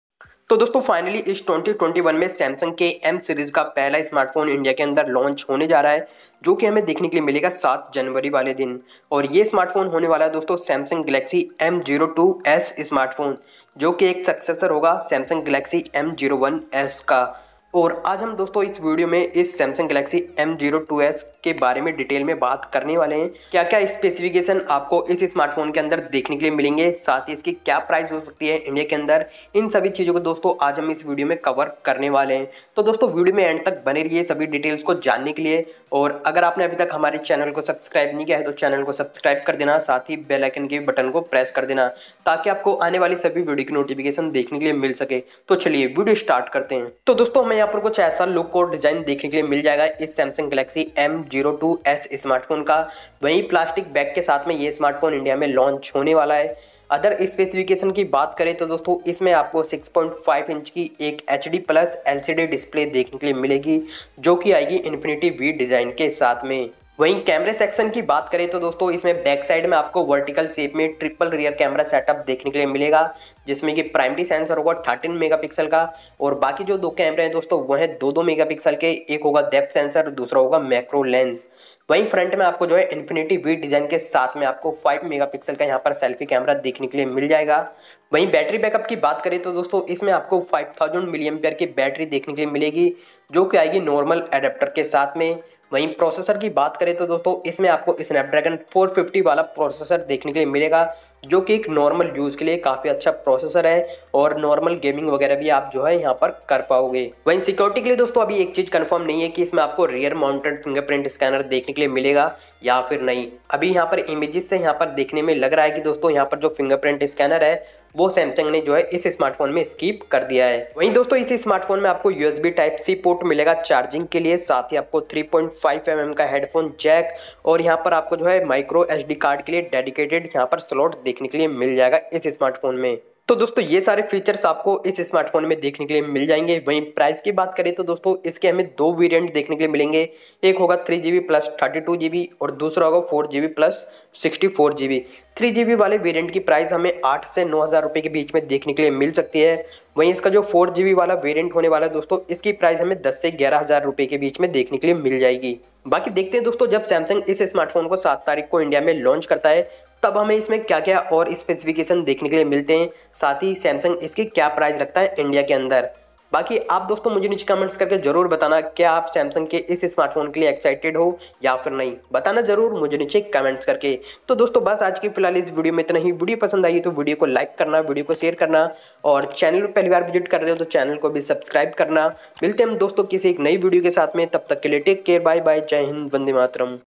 Samsung Galaxy M02s Launch Date Confirm | Full Specification | India Price | MaxUP
Namaskaar dosto Techno connect Channel par apka swagat hai or ajj hamne is video me Samsung galaxy M02s smartphone ke baare me discuss Kiya hai muje umeed hai ye video apko pasand ayega..